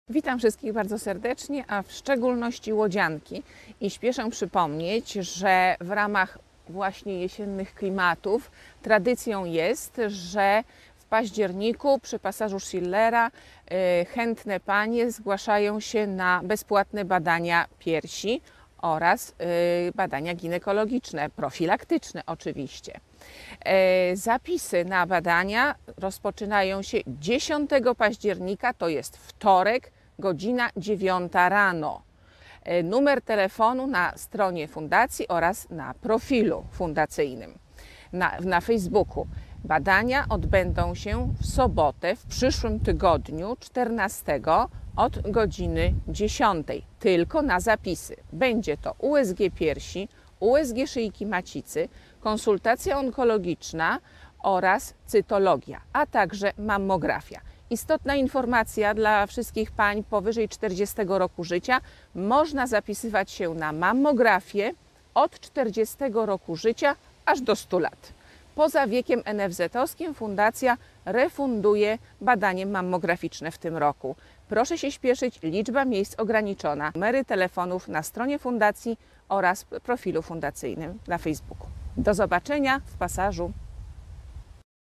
Zaproszenie na bezpłatne badania profilaktyczne które odbędą się 14.10.2023 r., godz.10.00
Zaproszenie na bezpłatne badania profilaktyczne które odbędą się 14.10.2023 r., godz.10.00 w ramach XIII Meetingu na Rzecz Walki z Rakiem Piersi i Szyjki Macicy